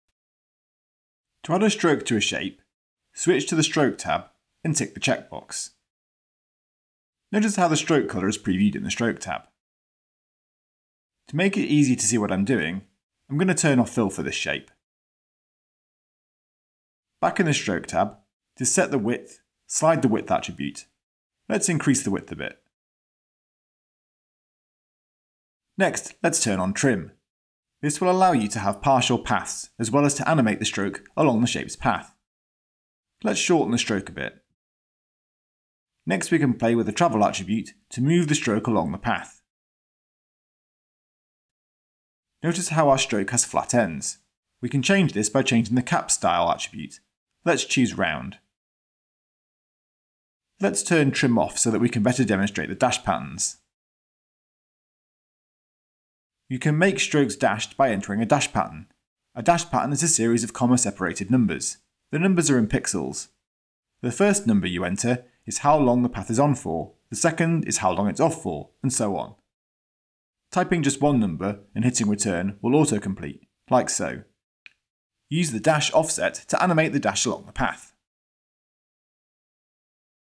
Getting Started | Adding and animating Strokes in Cavalry.
How to add a Stroke to your Shapes and set widths, dash patterns and trim the paths.

Created in Cavalry 1.0